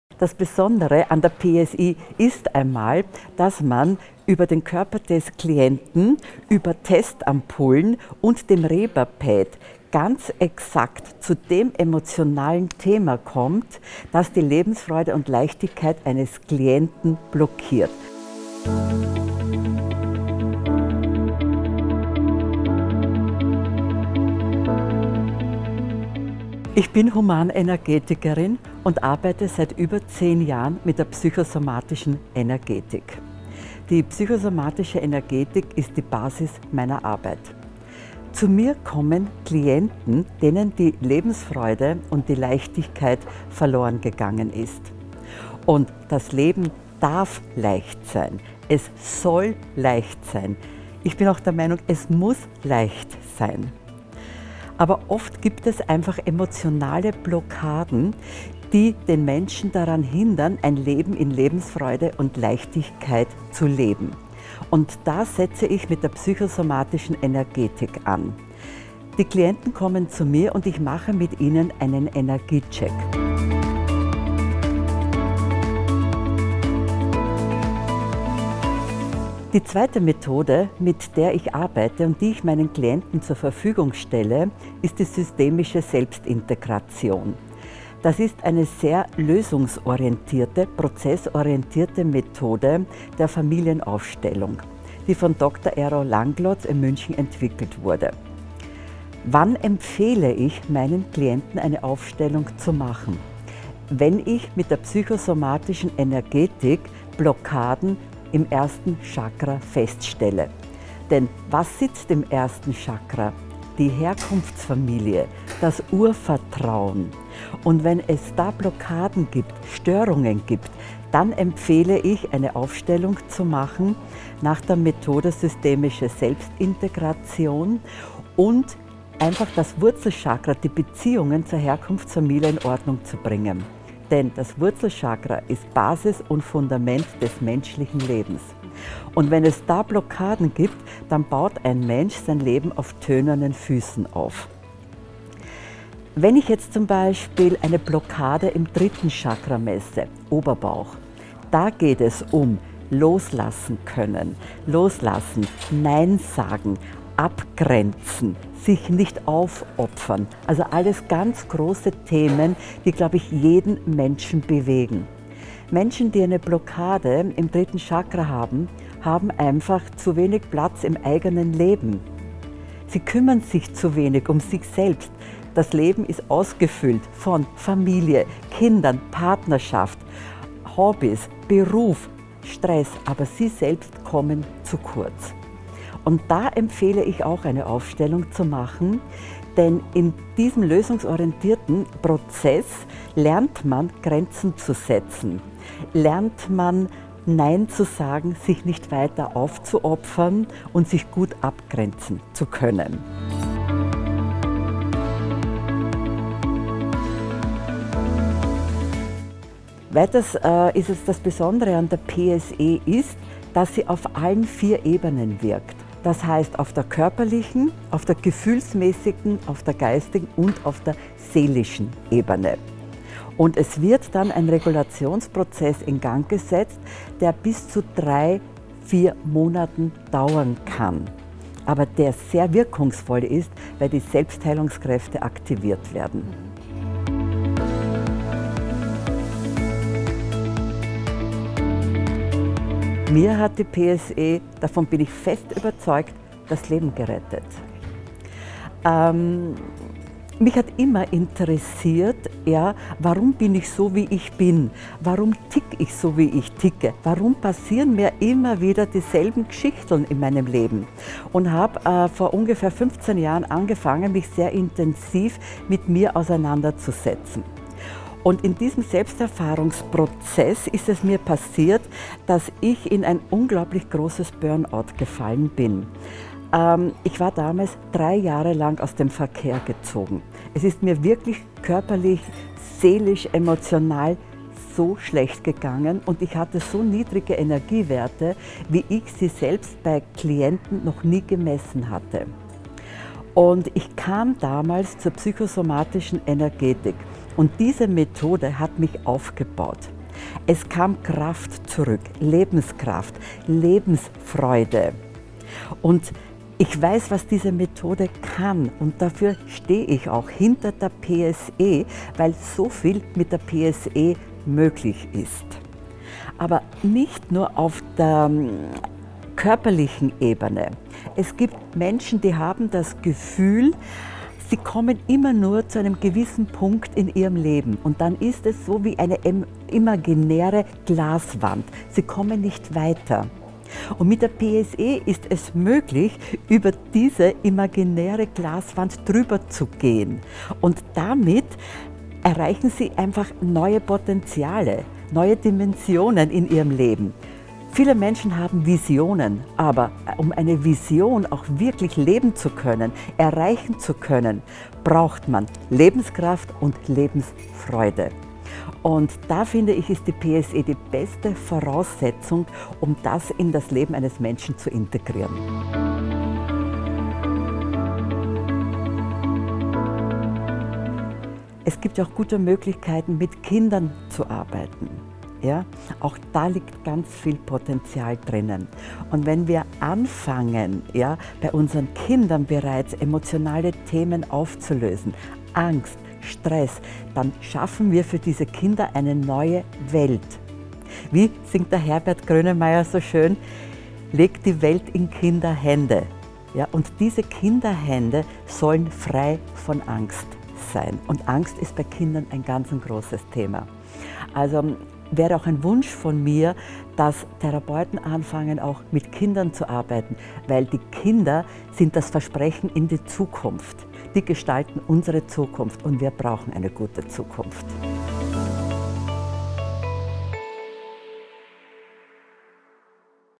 PSE & Familienstellen: Lebensfreude gewinnen mit Gesundheitsexpertin Christa Maly-Zach
0:25 Warum arbeiten Sie mit der PSE?
1:17 Mit welchen anderen Methoden kombinieren Sie die PSE?
3:26 Was ist das Besondere an der PSE?
4:02 Welche Erfahrungen haben Sie selbst mit der PSE gemacht?
6:05 Ist die PSE für jeden Patienten geeignet?

"Um eine Vision auch wirklich erreichen zu können, braucht man Lebenskraft und Lebensfreude.
Die PSE schafft die Voraussetzungen, um das in das Leben eines Menschen zu integrieren."

Humanenergetikerin Christa Maly-Zach arbeitet vor allem mit Klienten, denen die Lebensfreude verloren gegangen ist. Sie unterstützt dabei ein Leben in Freude, Leichtigkeit und mit Urvertrauen leben zu können. Dafür setzt sie die PSE als hilfreiche Methode ein, um einen sogenannten "Energiecheck" ihrer Klienten zu erstellen. Zusätzlich integriert sie das Familienstellen nach Dr. Langlotz, vor allem wenn es um Blockaden im Wurzelchakra geht.